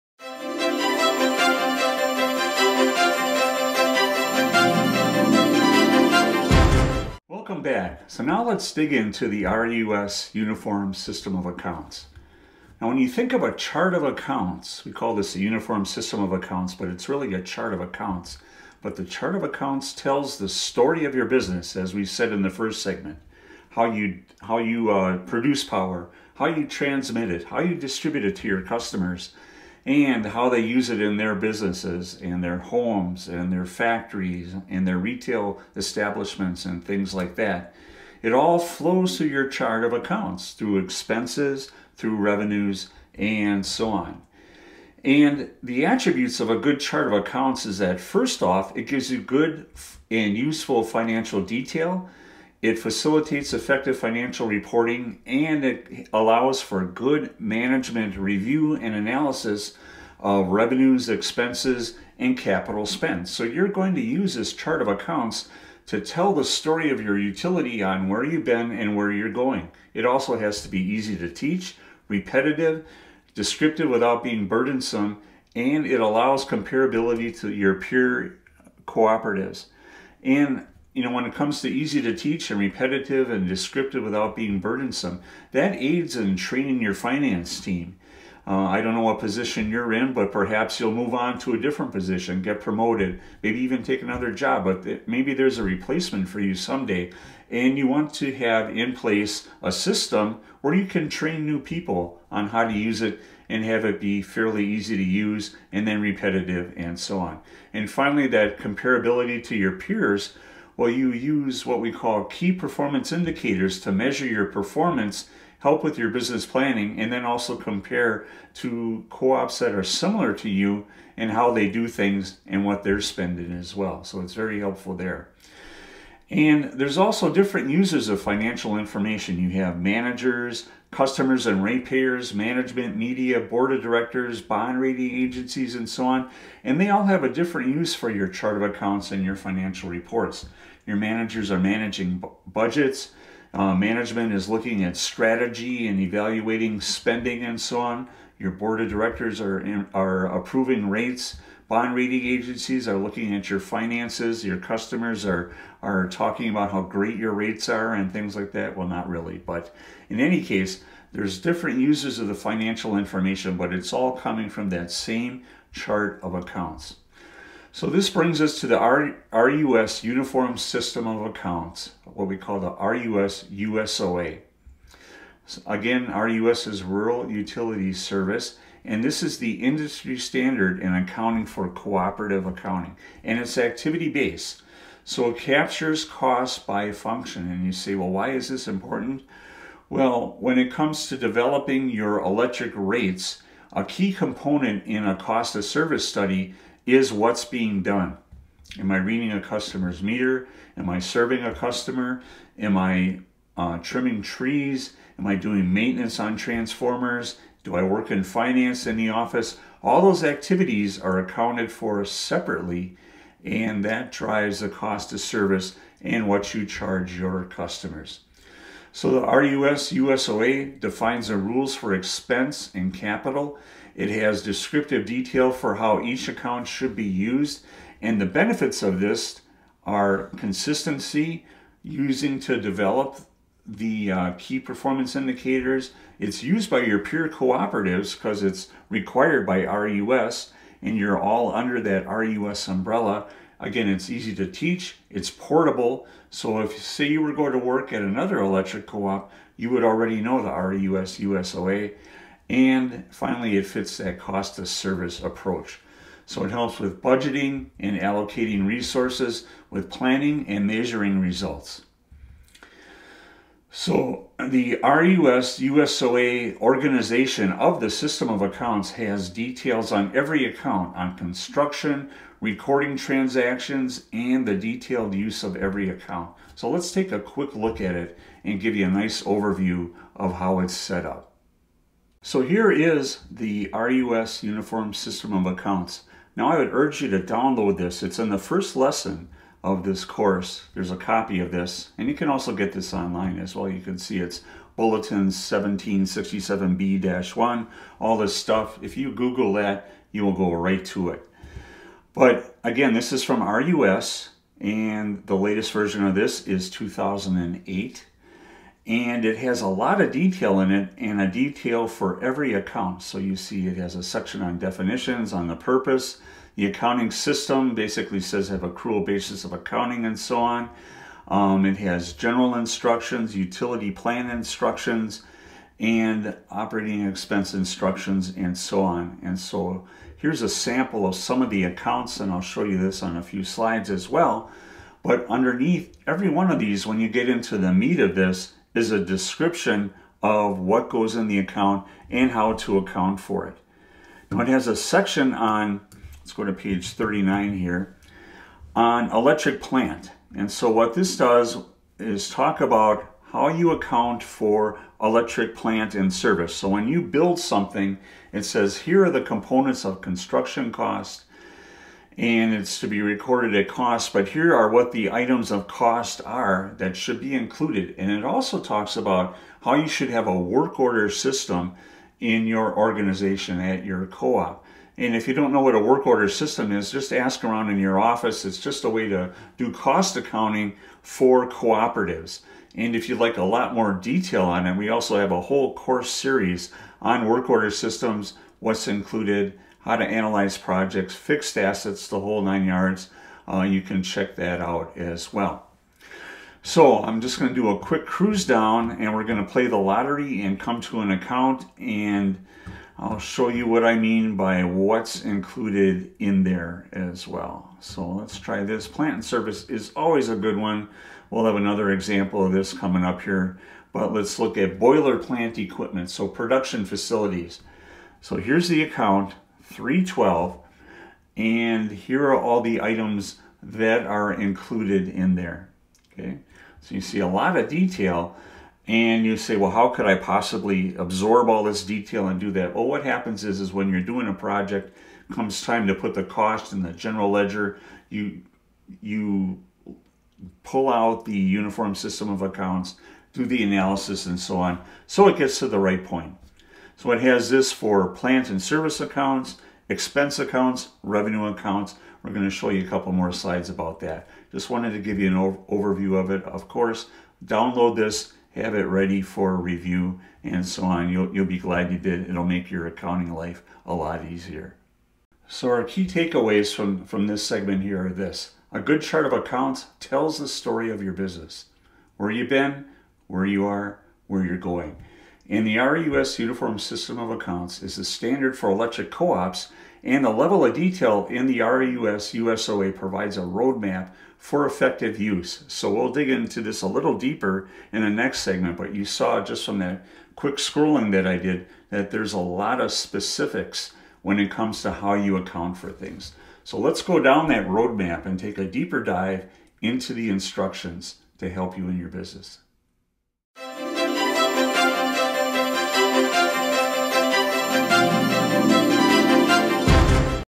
Intro to the RUS Uniform System of Accounts - snippet
Here's a taste of our course on the RUS Uniform System of Accounts. If you're in the electric cooperative finance areas, the RUS USOA is your accounting foundations. Here we provide some background.